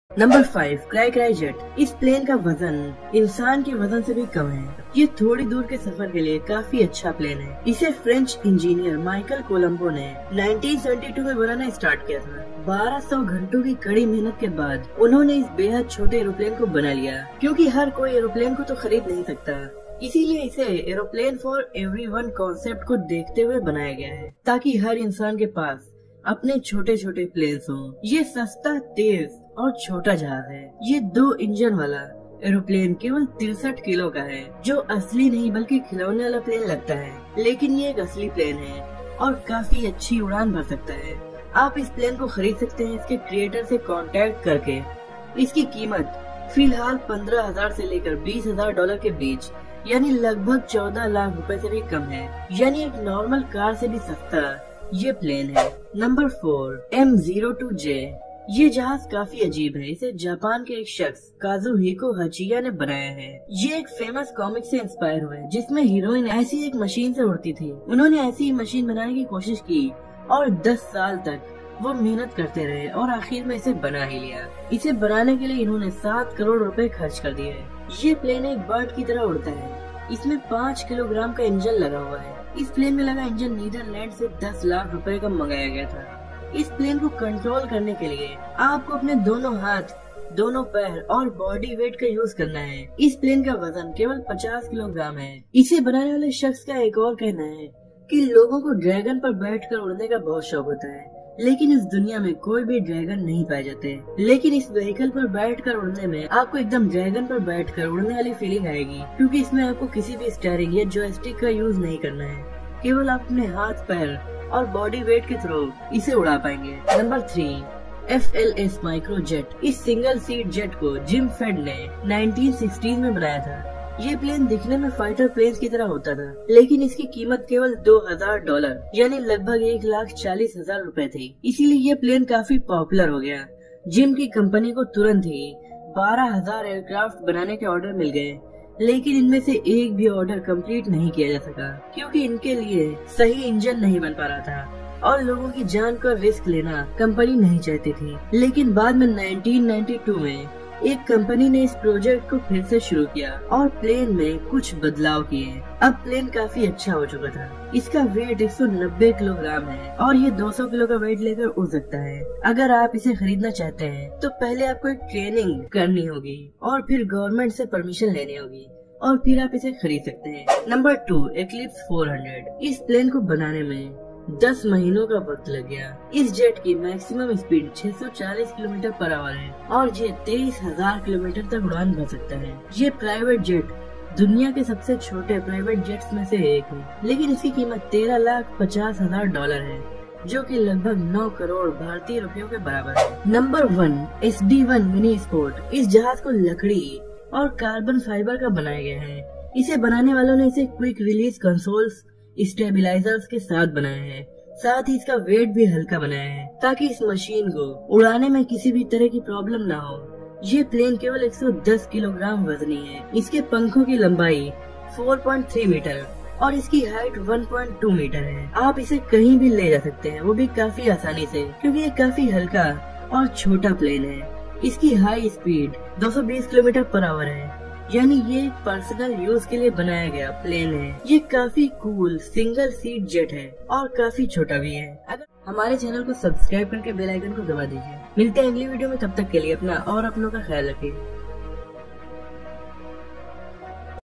5 दुनिया के सबसे छोटे हवाई जहाज l top 5 smallest plans in the world
Hello my dear friends,in this video I'm going to show top 5 world smallest plans in the world